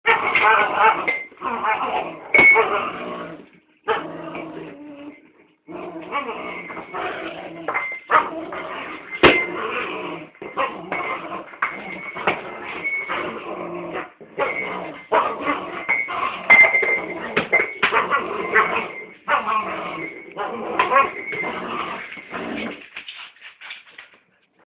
My Cocker Spaniel playing with a bowl
He is playing with his bowl.